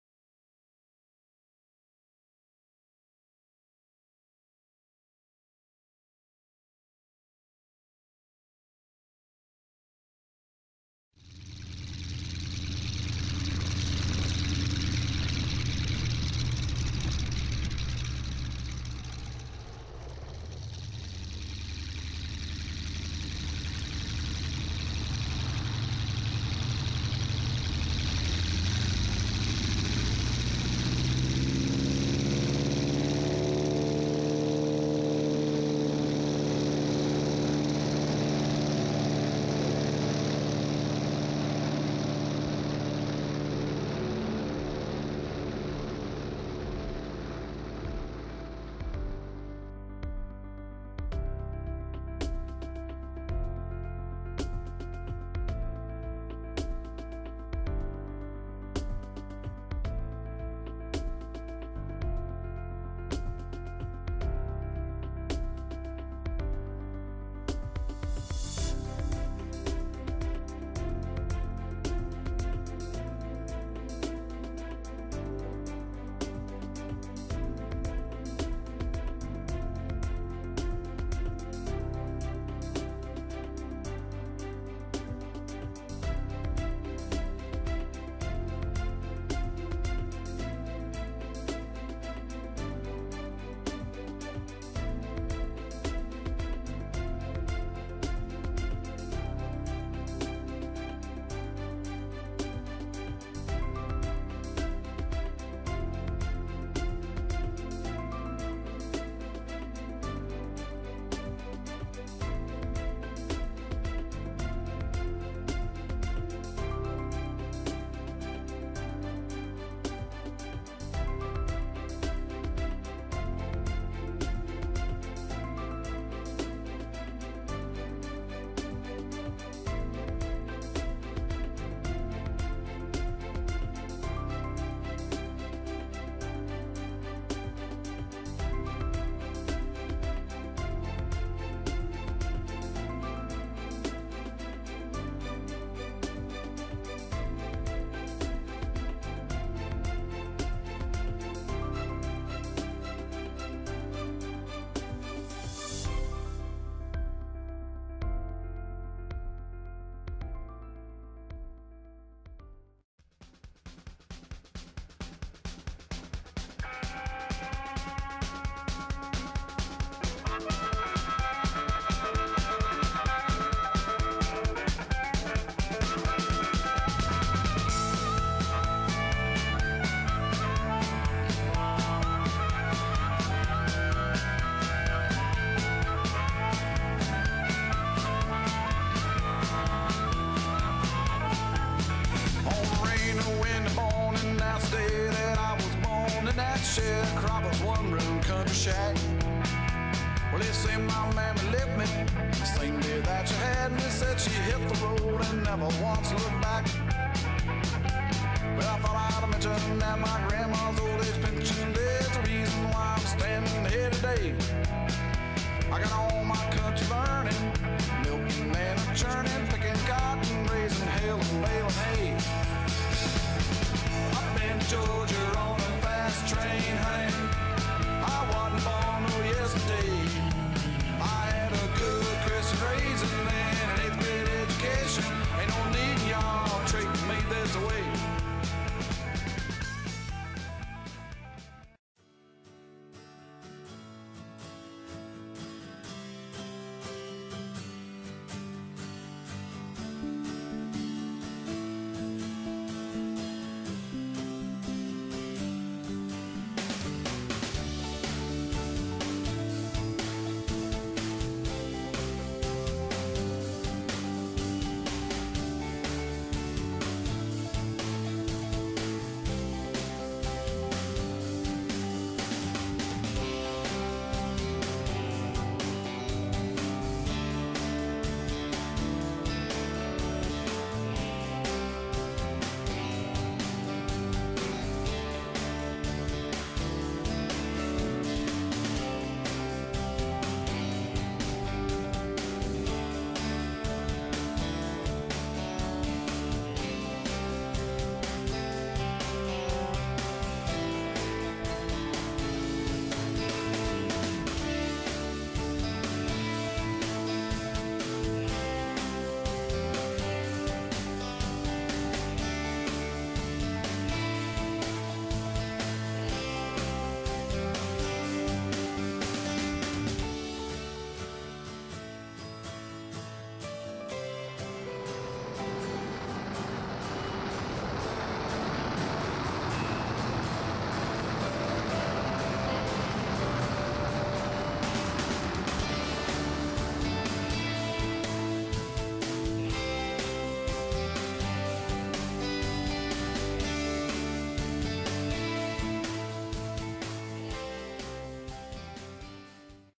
OBX Air Tour Adventures: Craig flys a T6 Texan warbird over the OBX!
Looking us up for some fun in our T6 Texan Sweet Sophia, Craig joins us for an aerobatic flight over the Outer Banks of North Carolina.  Departing Manteo Airport, they head straight to the ocean to enjoy a few aerobatics before continuing the tour along the ocean.  Many thanks for flying with us Craig!